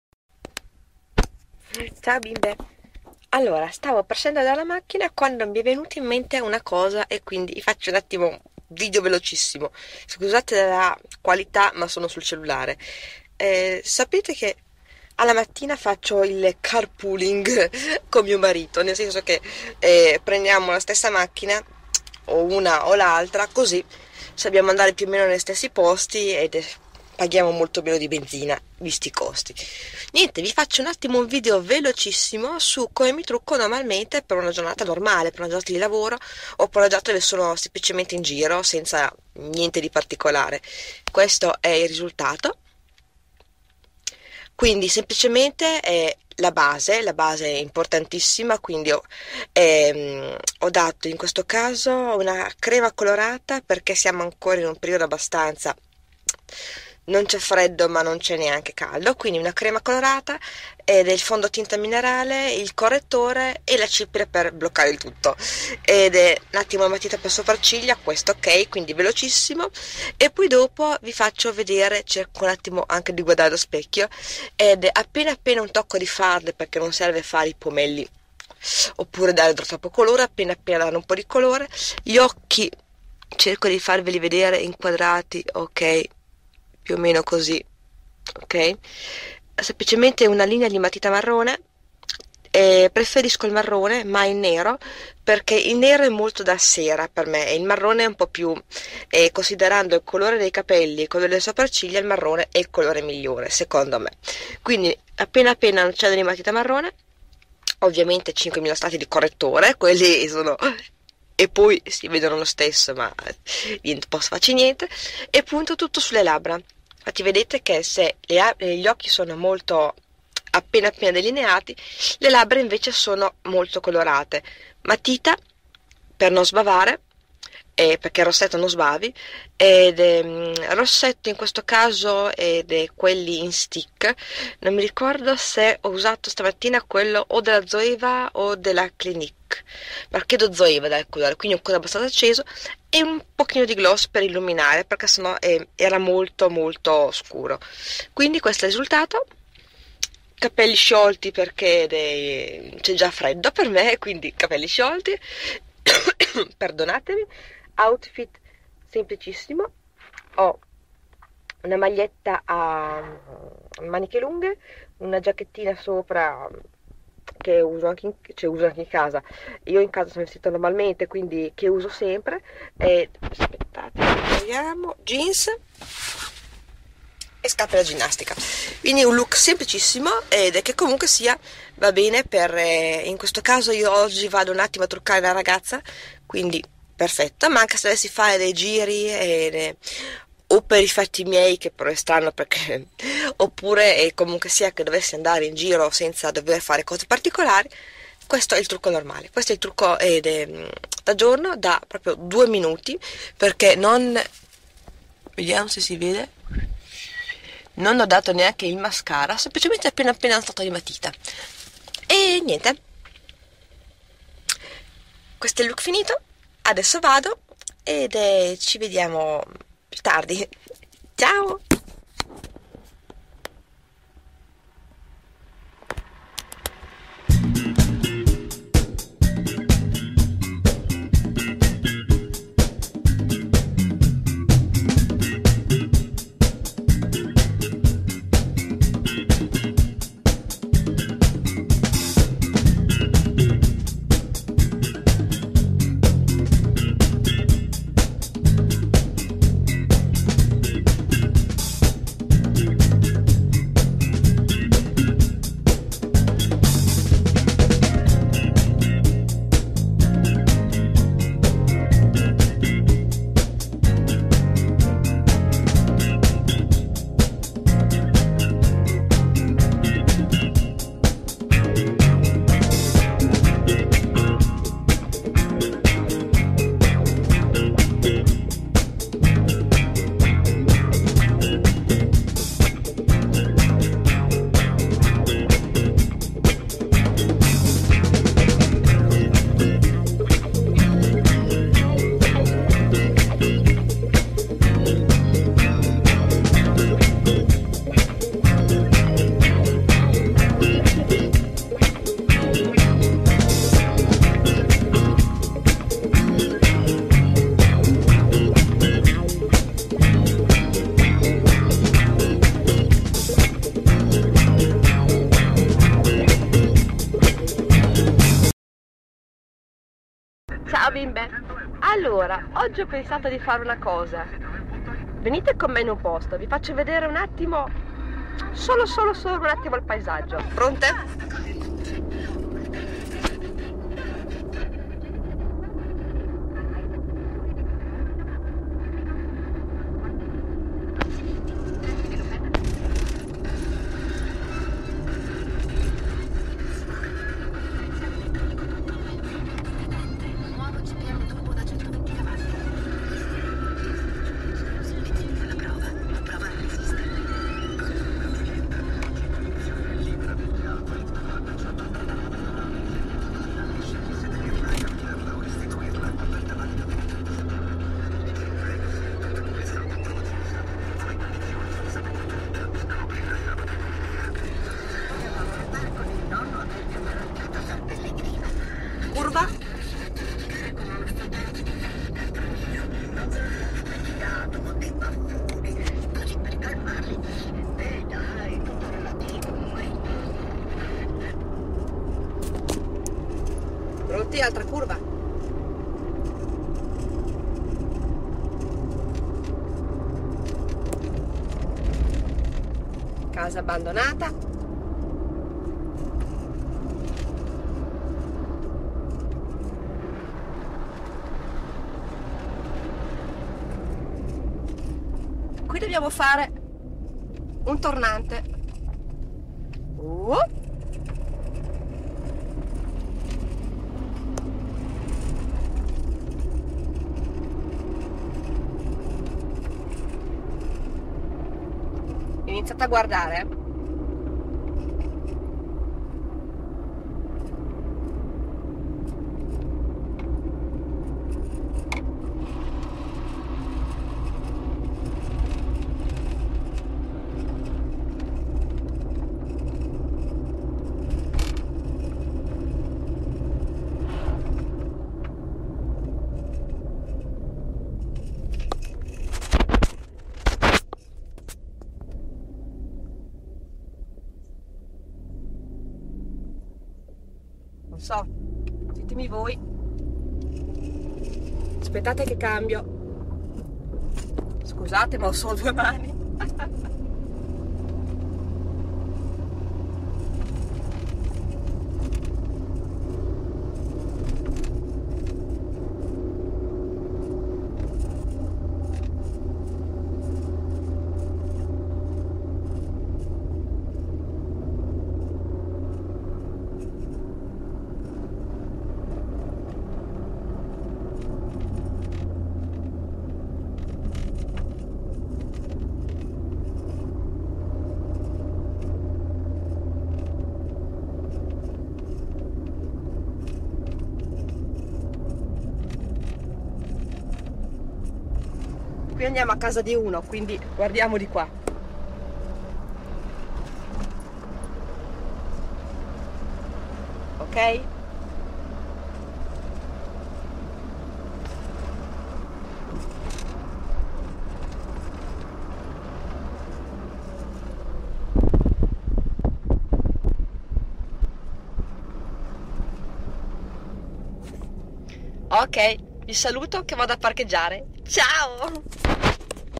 Makeup per Tutti i Giorni ... più on the road finale
Attenzione! Ho registato la parte finale su una strada che conosco bene e di pochissimo passaggio.
NON registrate MAI mentre giudate, mi raccomando!

°Oo  LEGGIMI !!! oO°